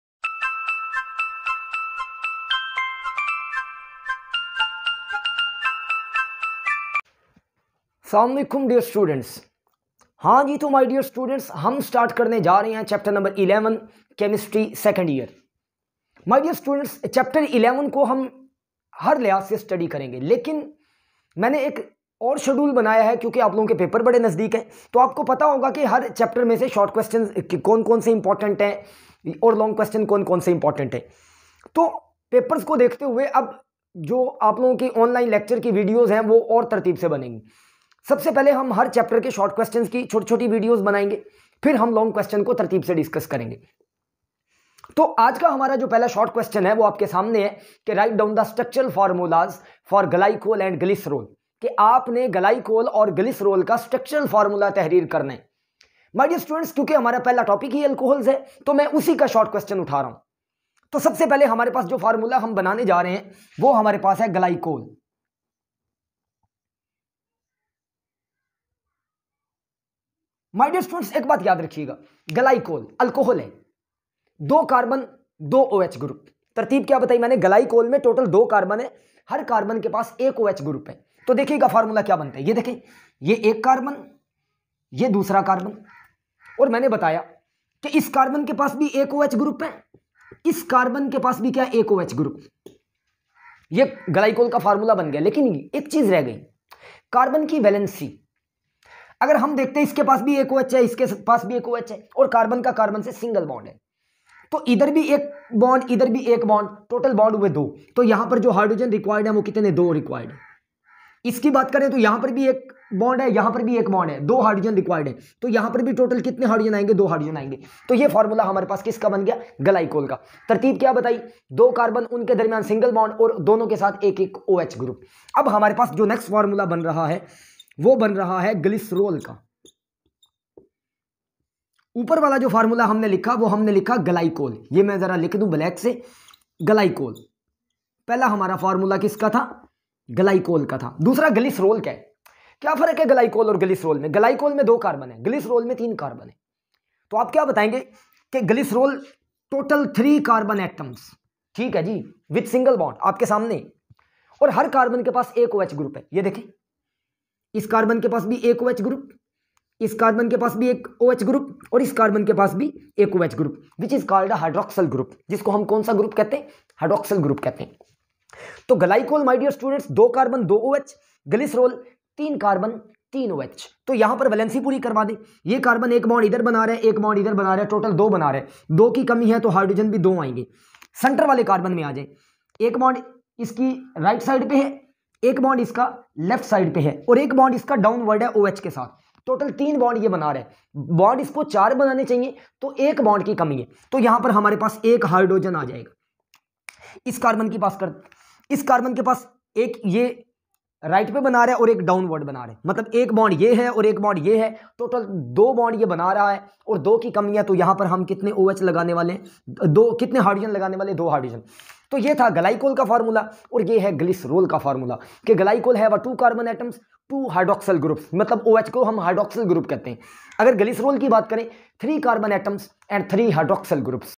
How to Write the Chemical Formula of Glycol and Glycerol.||advance knowledge
Topic.
            How to write the chemical formulas of glycol and glycerol.